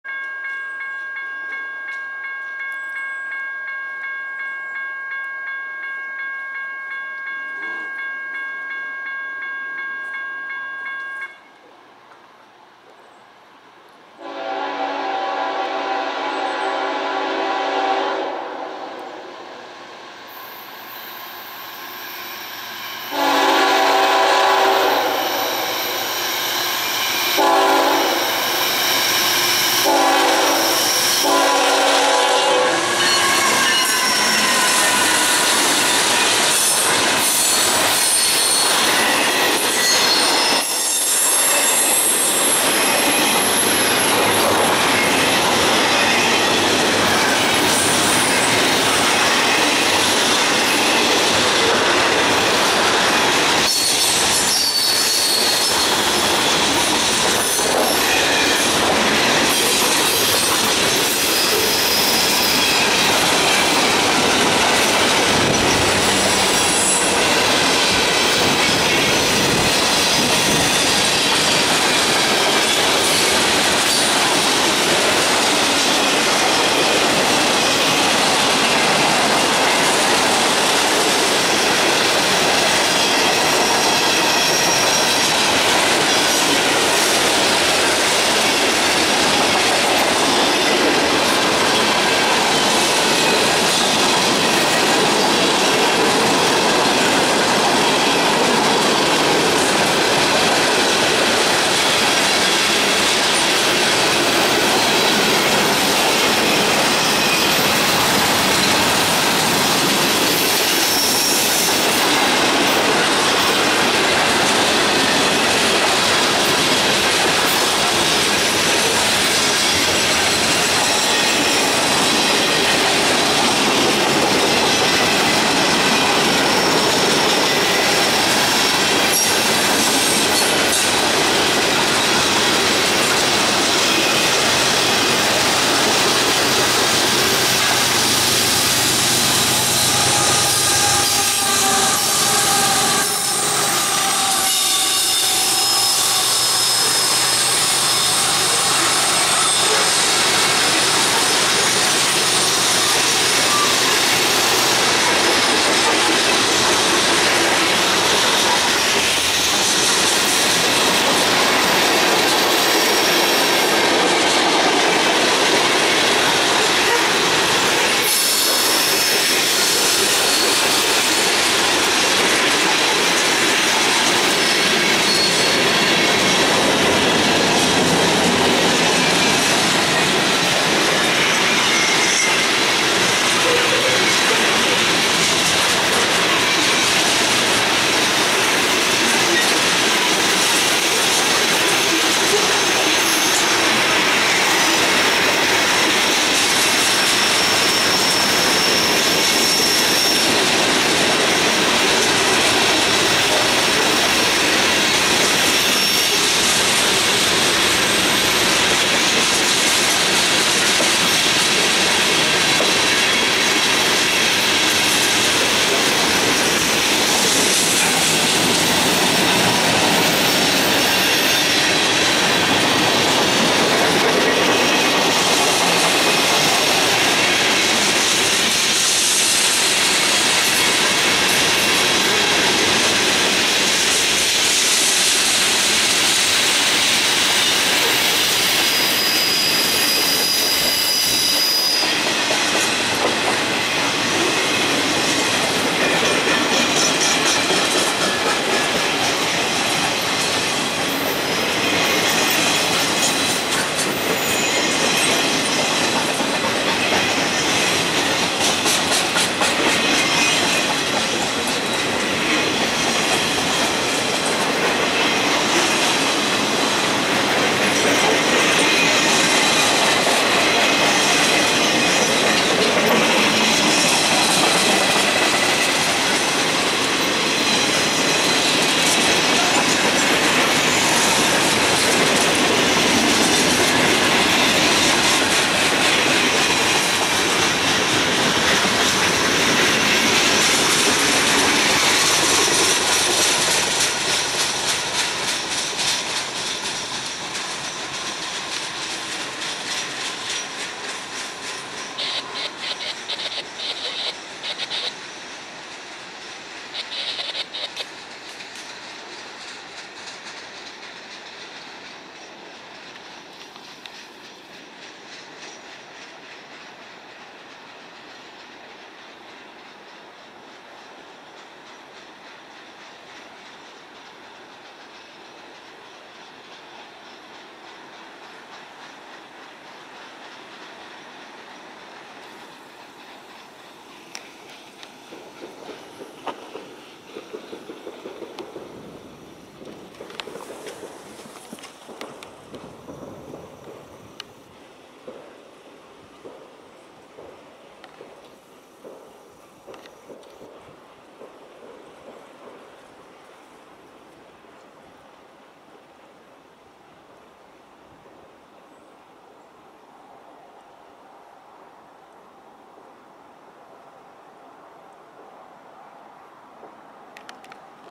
CSX Mixed Train With Double Stacks and MOW Equipment
CSX mixed manifest freight train Q415 heads south through the Colemans Mill Road grade crossing in Ruther Glan, Virginia, with a lot of maintenance of way equipment. Locomotives: CSX 3474(GE ET44AH), CSX 3238(GE ET44AC), &  CSX 3127(GE ES44AH).